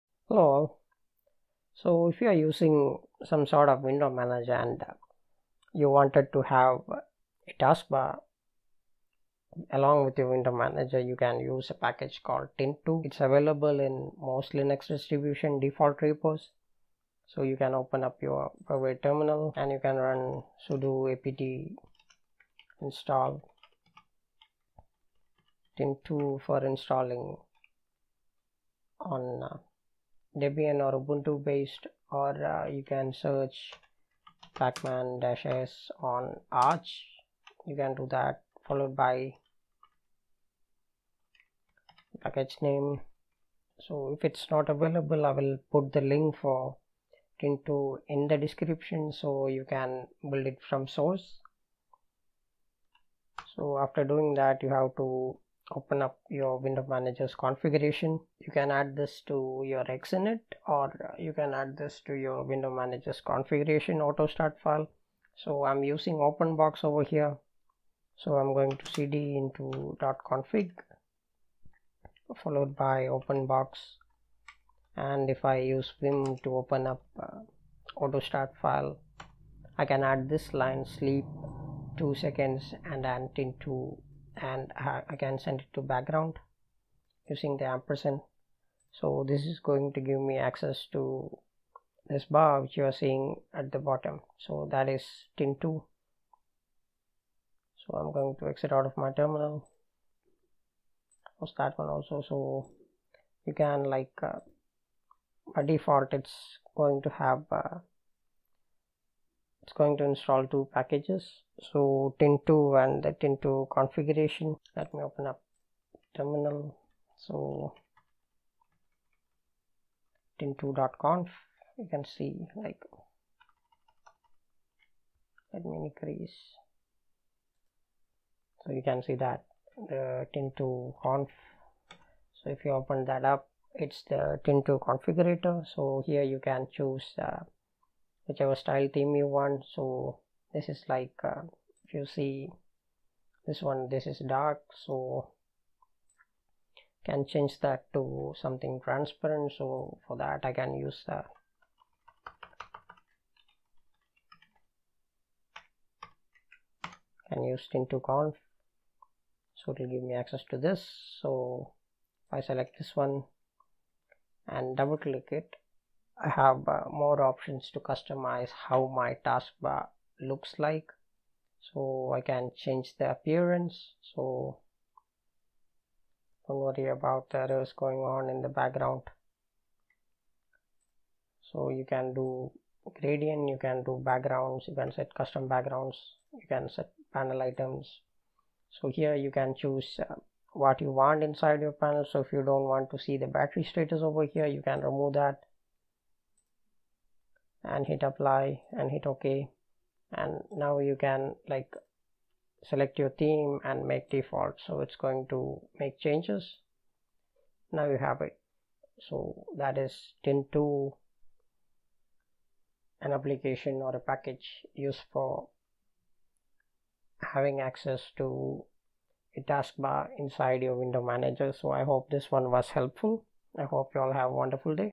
Tint2 - The taskbar for your Window Manager
A basic usage of tint2 on Openbox its available on most Linux distributions by default